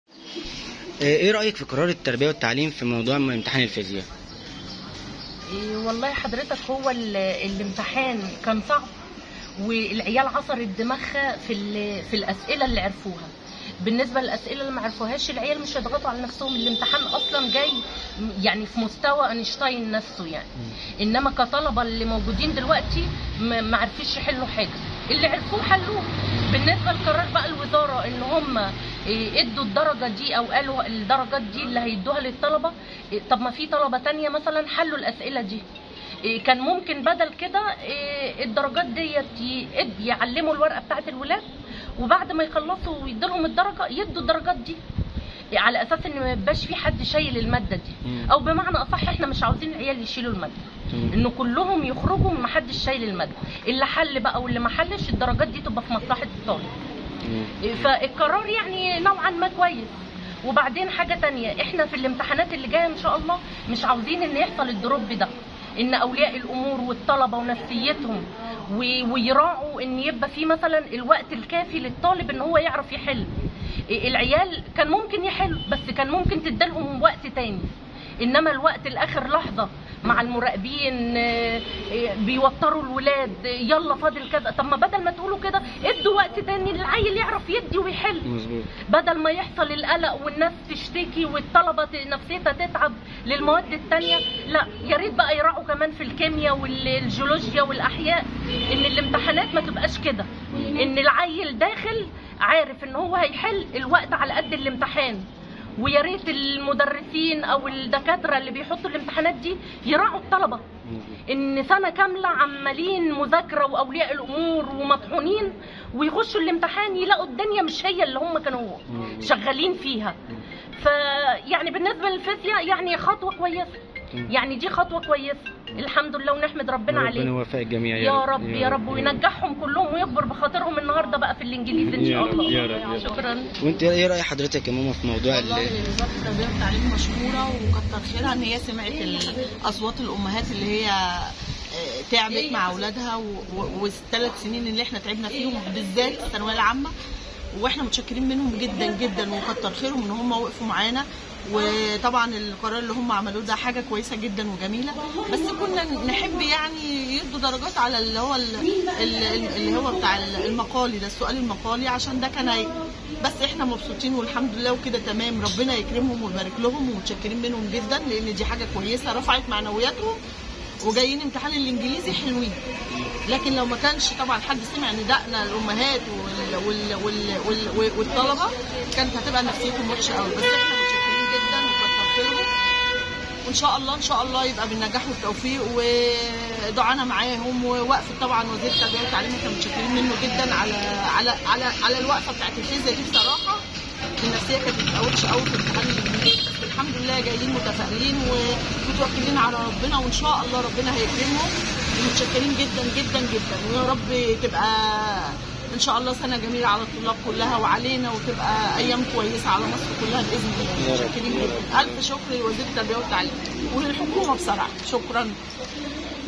أولياء الأمور: امتحان الفيزياء جه صعب وقرار التعليم في مصلحة الطلابتصوير/ مكاريوس كرم
أولياء الأمور: امتحان الفيزياء جه صعب وقرار التعليم في مصلحة الطلاب
تصوير/ مكاريوس كرم